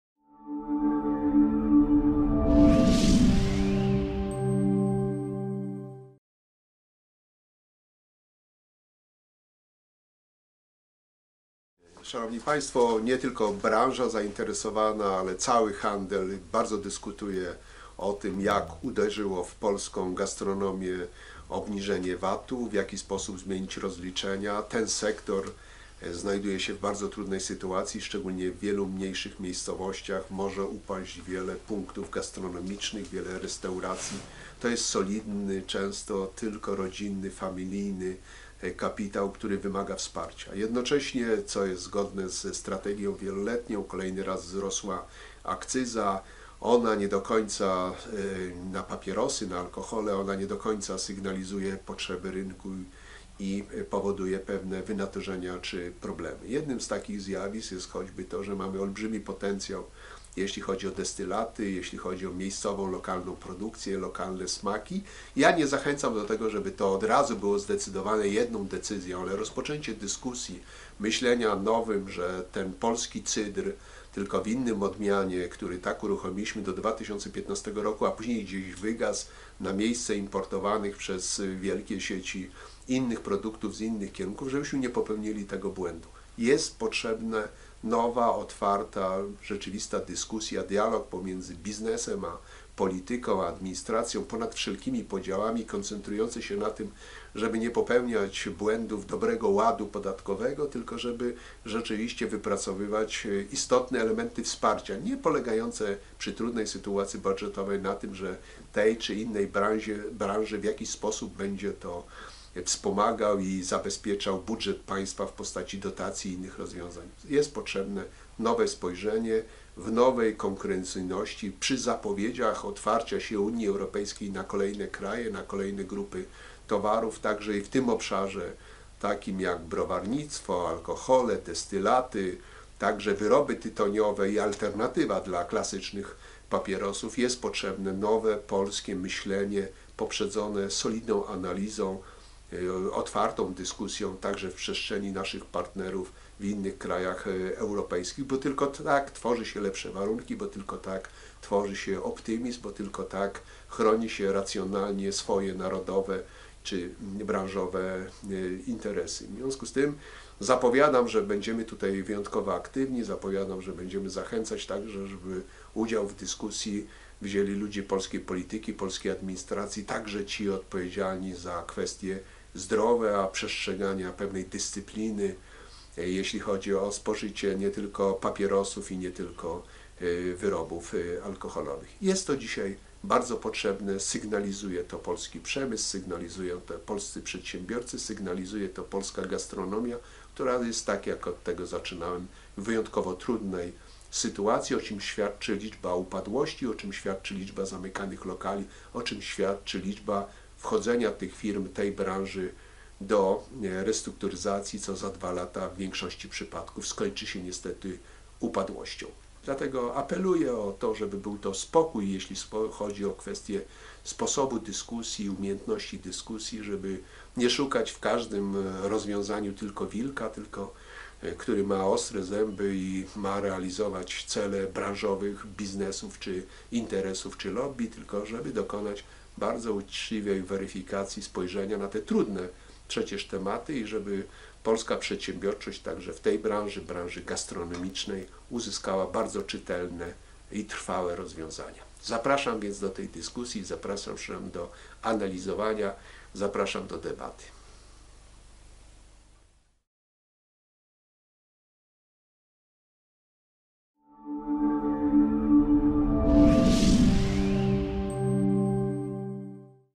Uwolnić destylaty! Janusz Piechociński
Uwolnić destylaty! - postuluje Cezary Kaźmierczak, prezes Związku Przedsiębiorców i Pracodawców. Na zachodzie Europy wytwarzanie destylatów jest legalne. Regiony chlubią się swymi oryginalnymi produktami. Nie ma sensu, żeby  w Polsce tkwiło w szarej strefie czy wręcz było nielegalne. Najwyższy czas uwolnić ten rynek.  Co na to Janusz Piechociński?